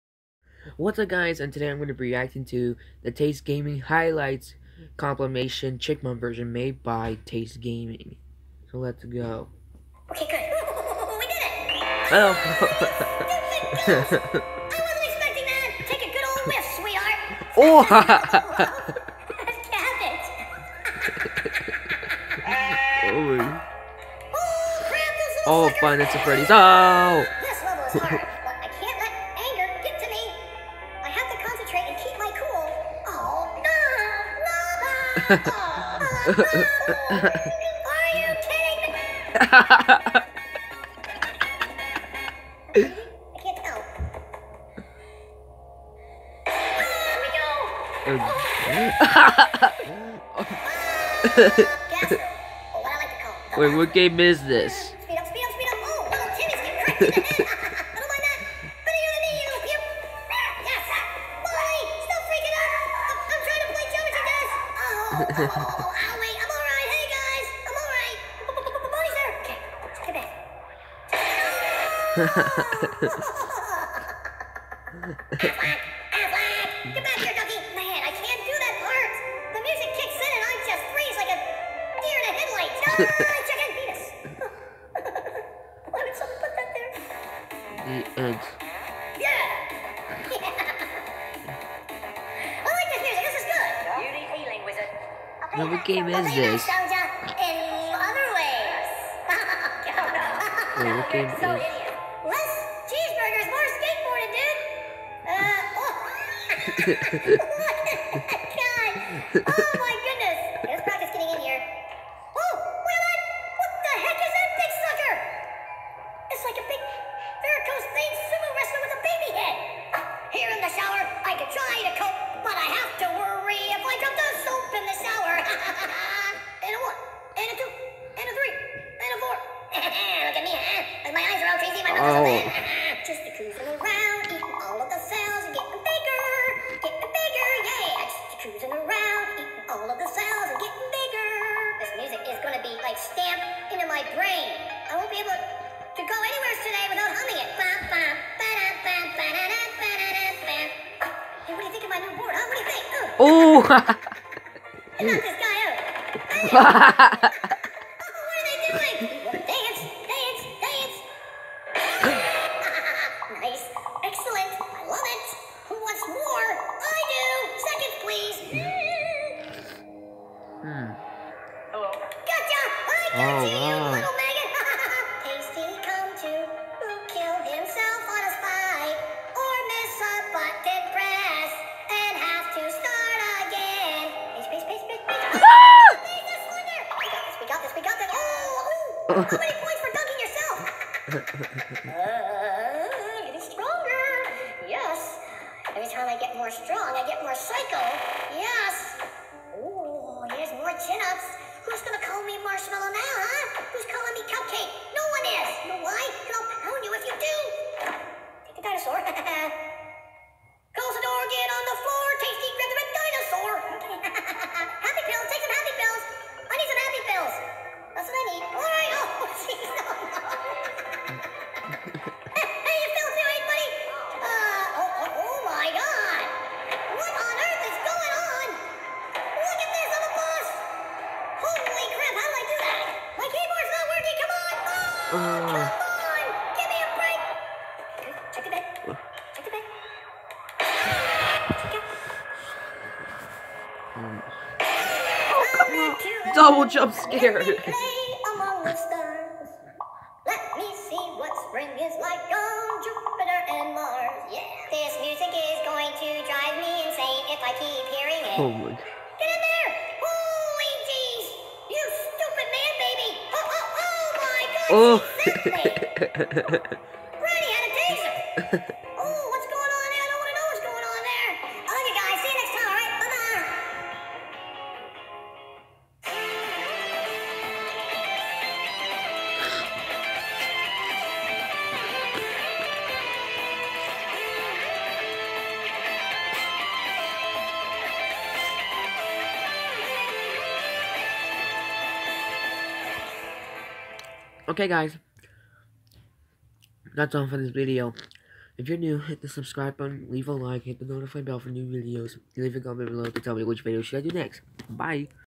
PowerSteve65 reacts to taste gaming highlights chipmunk edition
this is my thirteenth reaction video in this I react to taste gaming highlights chipmunk edition